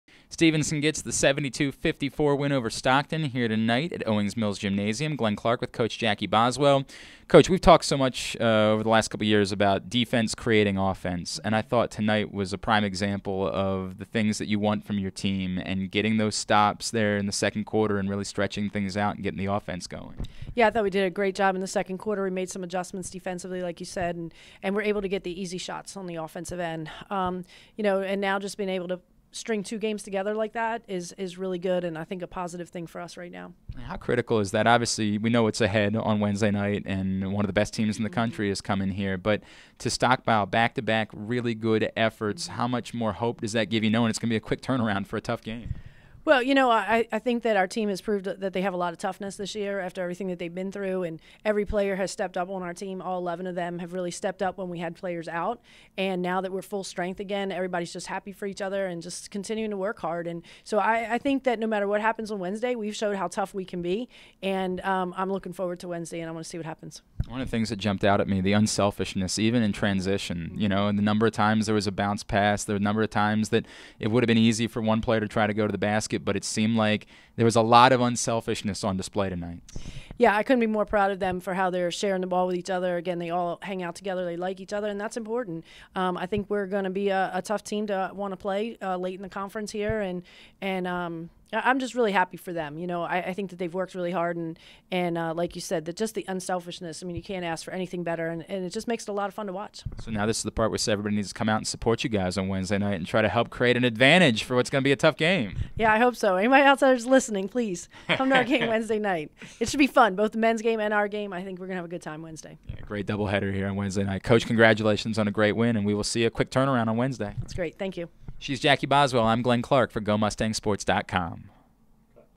Women's Basketball vs Stockton Postgame with Jackie Boswell
Stevenson head coach Jackie Boswell talks about the team's victory over Stockton this evening.